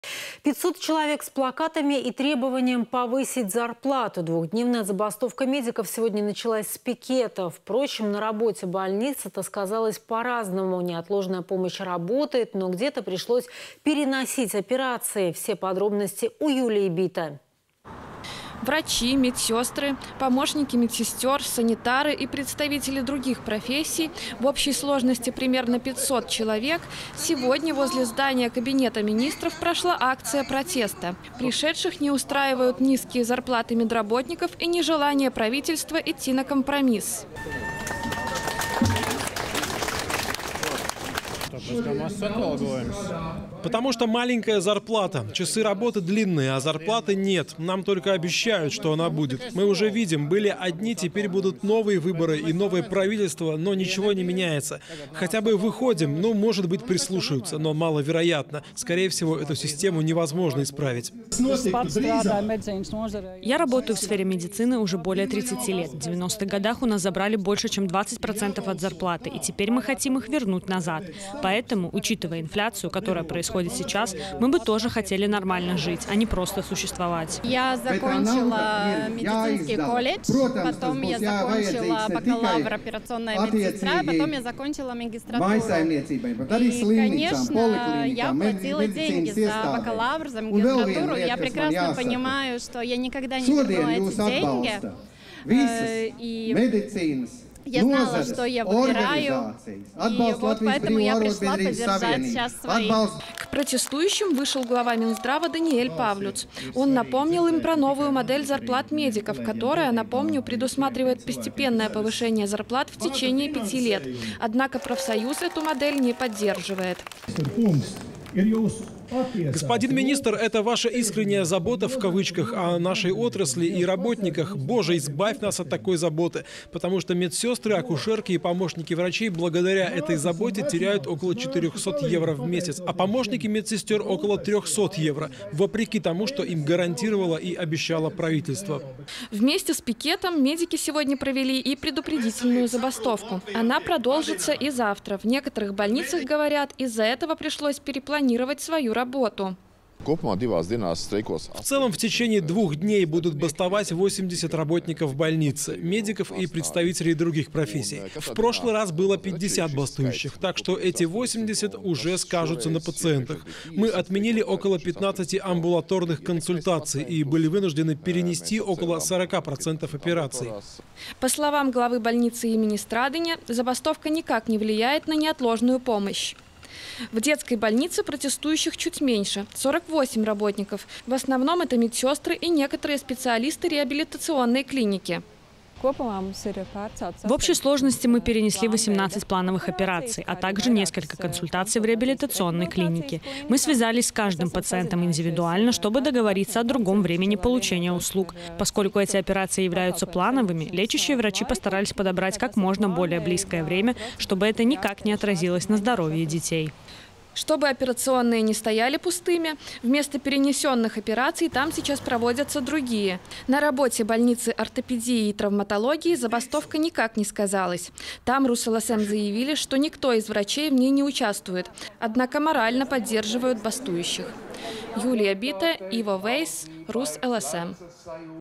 Медики начали забастовку и вышли на пикет
500 человек с плакатами и требованием повысить зарплату. Двухдневная забастовка медиков сегодня началась с пикета.  Впрочем, на работе больниц это сказалось по-разному. Неотложная помощь работает, но где-то пришлось переносить операции. Подробности у Юлии Бите.